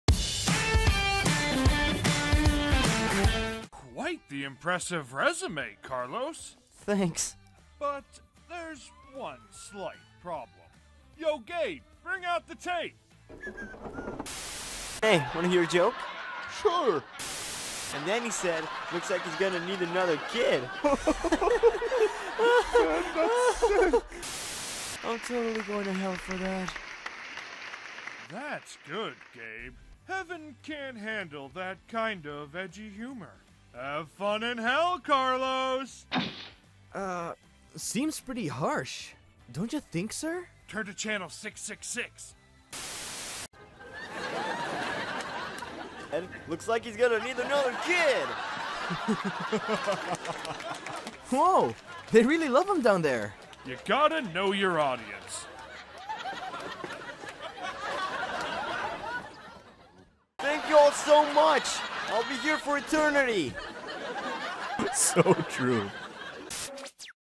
Adventures of God - Joke (2017 Comic Dub Competition Winner)
This is episode 68 of the comic Adventures of God
and also an application for the Kyotosomo Comic Dub Competition!
(this will be uploaded only if  i place in the top 3 of the competition :D)

Black guy: me...